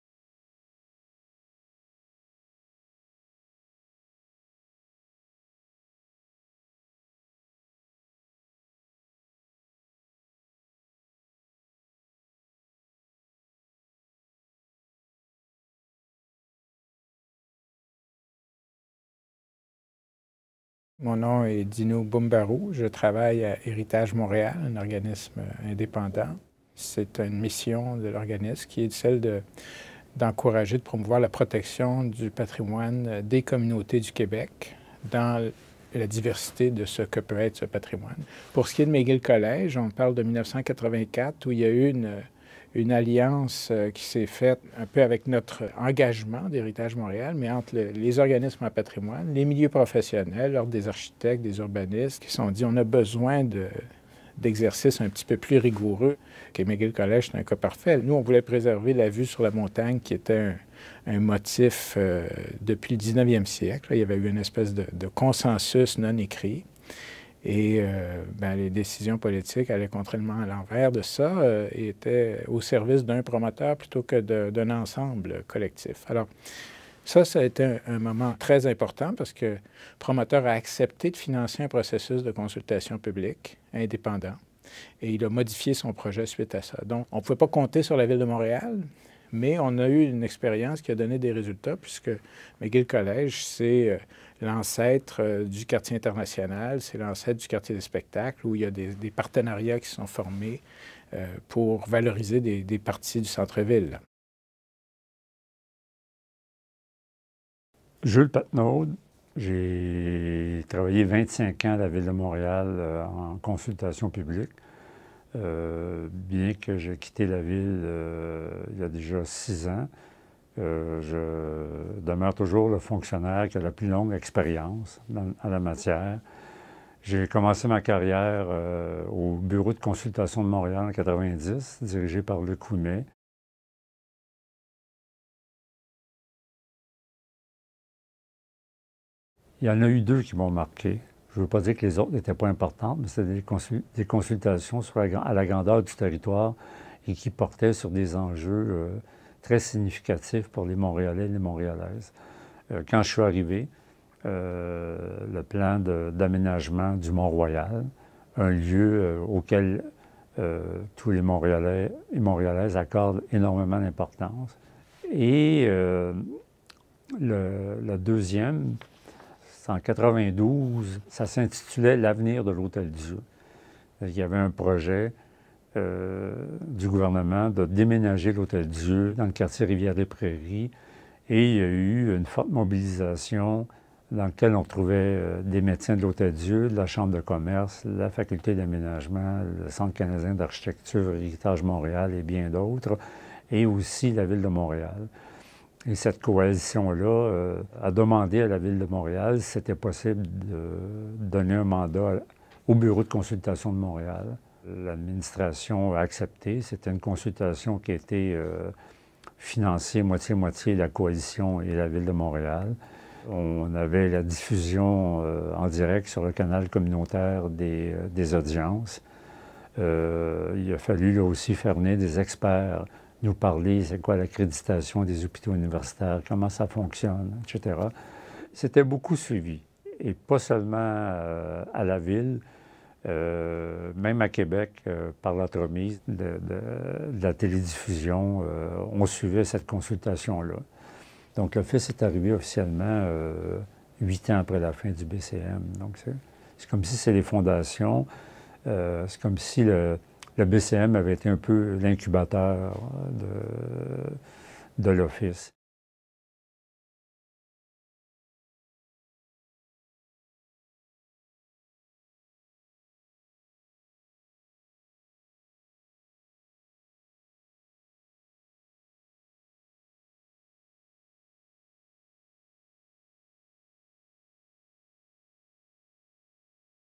Des revendications des années 1960 à la création de l’Office de consultation publique de Montréal
La volonté des Montréalaises et Montréalais d’être consultés dans les débats sur l’aménagement s’exprime tôt dans l’histoire de la ville, et de plus en plus fermement au milieu du XXe siècle.